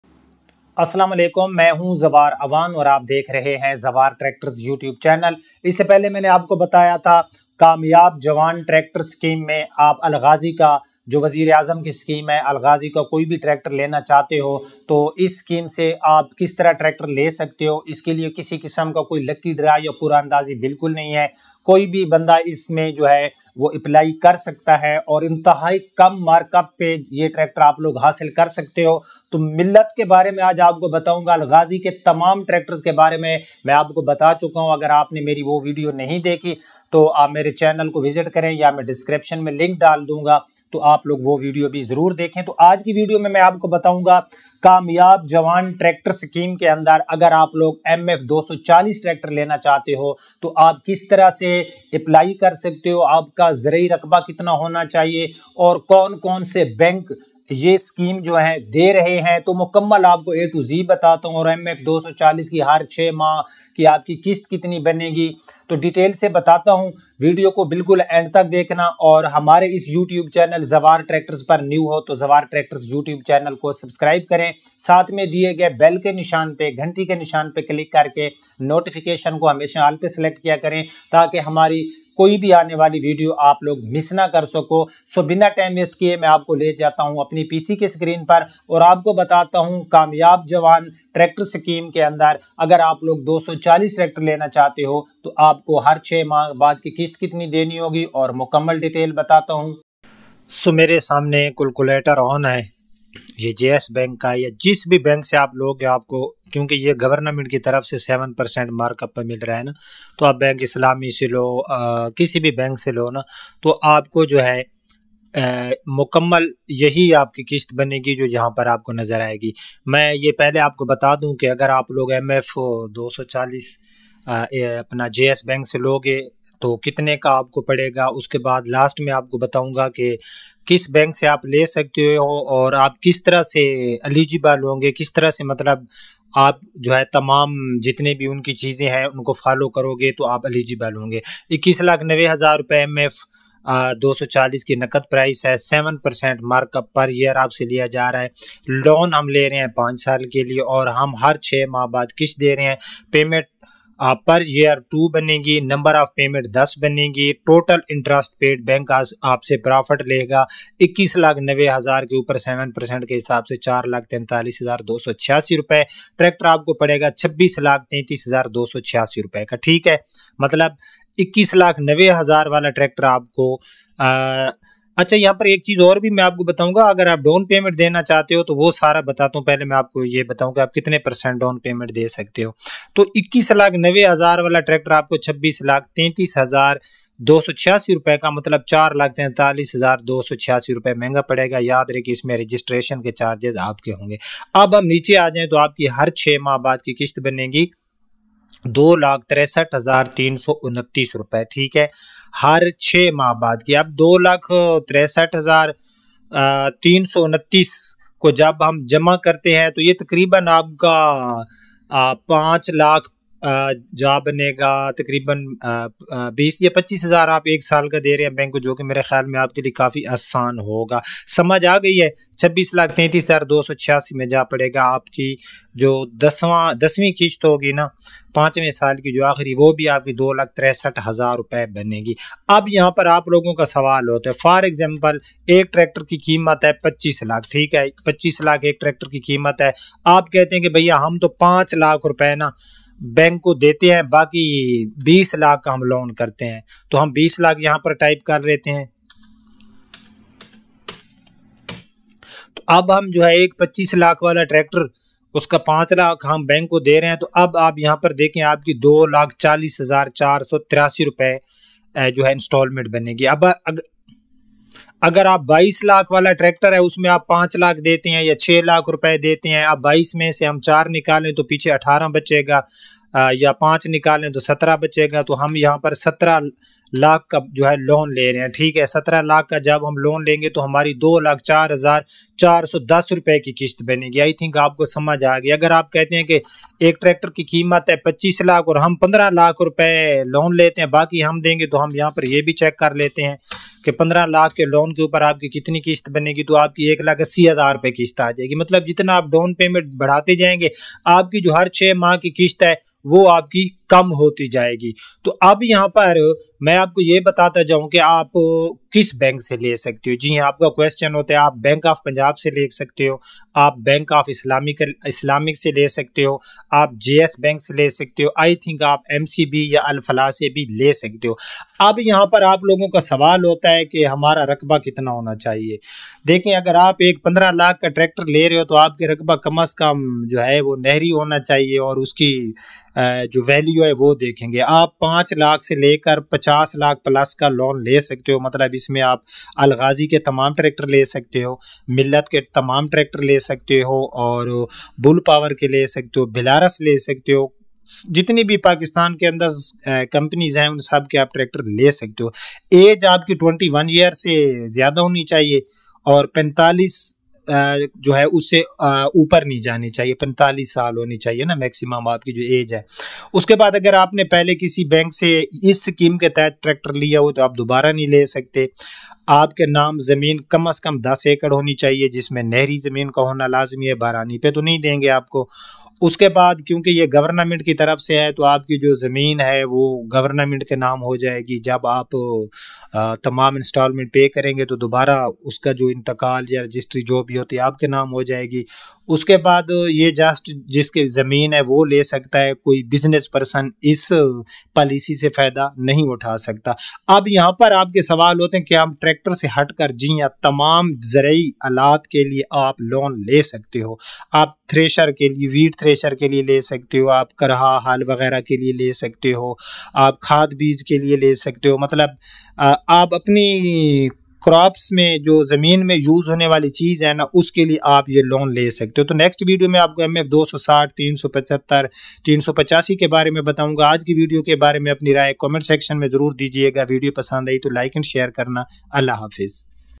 Wazir E Azam kamyab jawan program Sy Mf 240 Tractor 2024 Mukmal Palan
Buy tractors at only 7% markup from Prime Minister's Successful Young Tractors Scheme